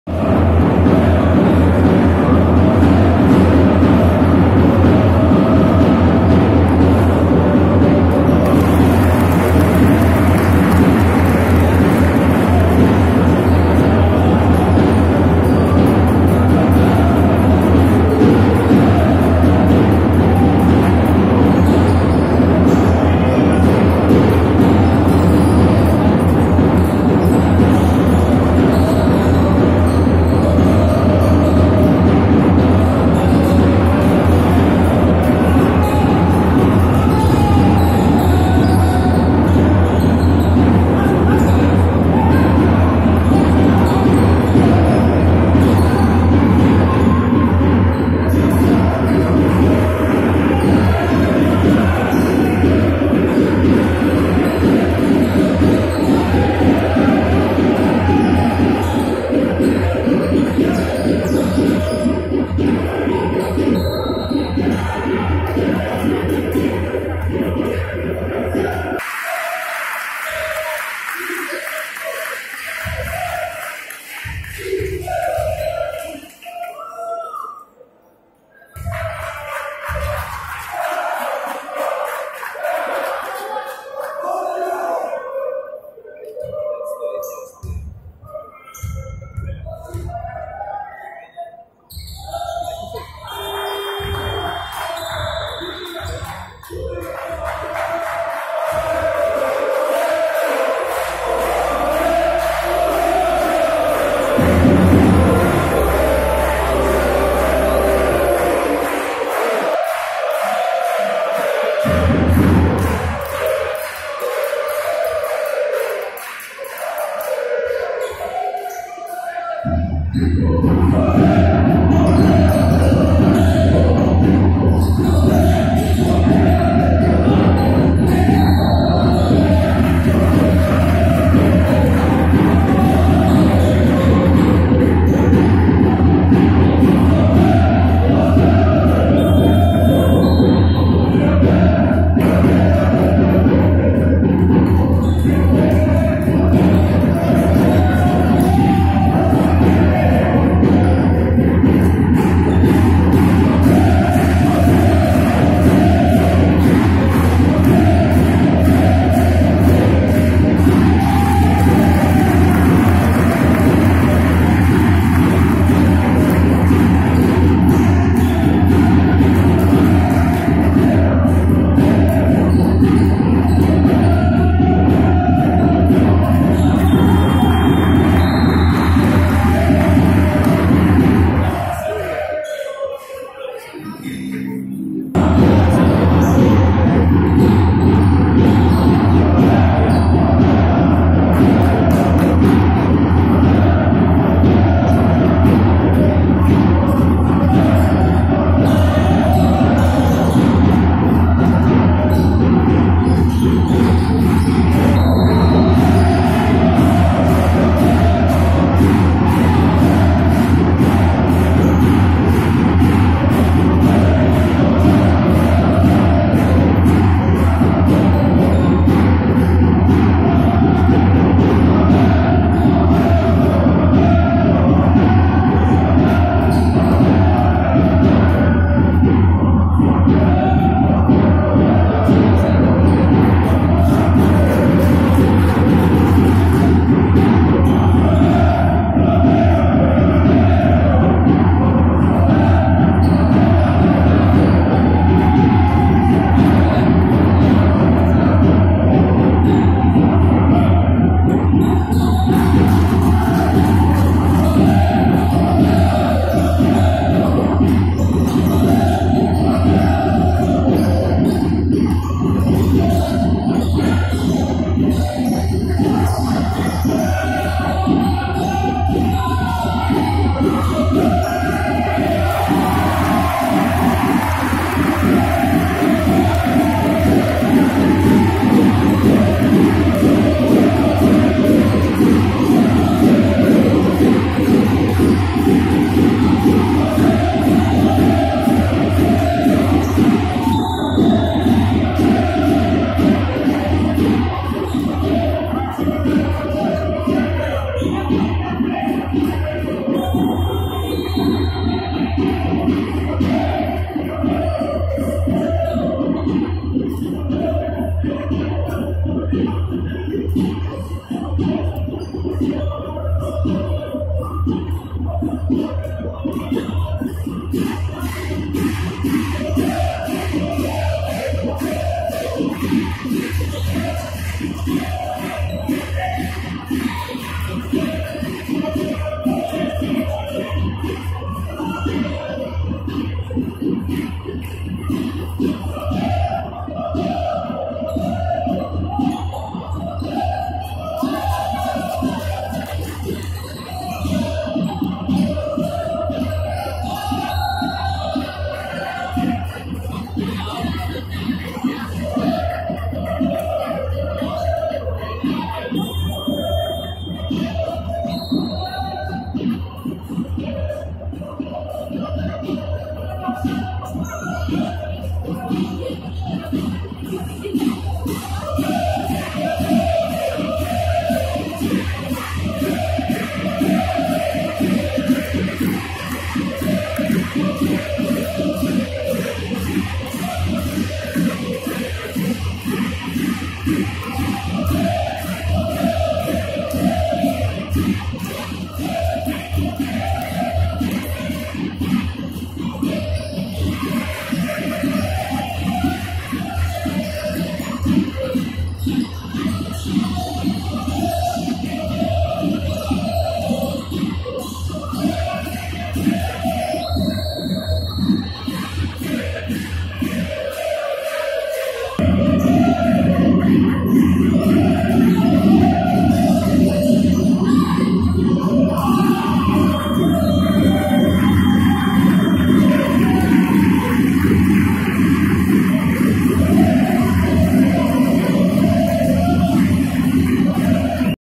ΒΙΚΟΣ ΠΑΣ ΓΙΑΝΝΙΝΑ vs Κ.Σ. ΓΕΦΥΡΑΣ 77-70
Τα τελευταία 2 λεπτά του αγώνα